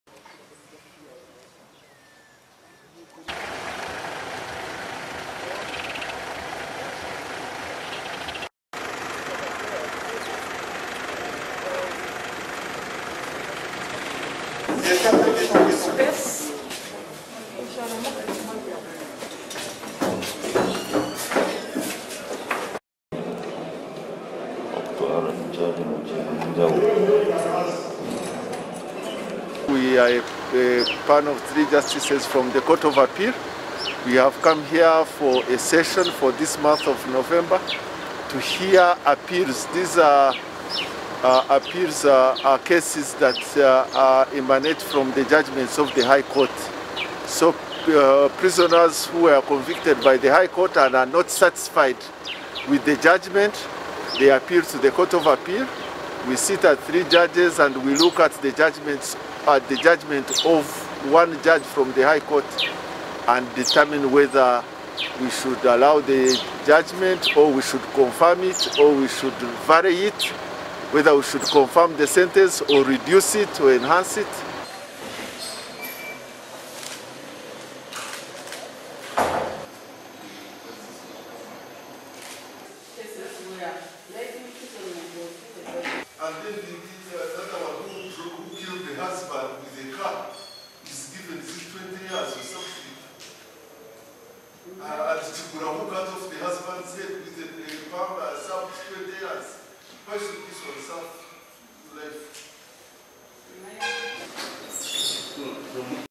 Judges in Kabarole hear cases, with most being sexual offenses
A panel of three justices of court of appeal has today pitched a camp in the Fort Portal court of appeal to start hearing a batch of cases. According to the cause list that was issued by the court of appeal deputy registrar Ayebare Tumwebaze that has 40 criminal appeal cases, those convicted on aggravated defilement cases are 20, aggravated robbery 4 cases, murder 15 cases and one rape case. NTVNews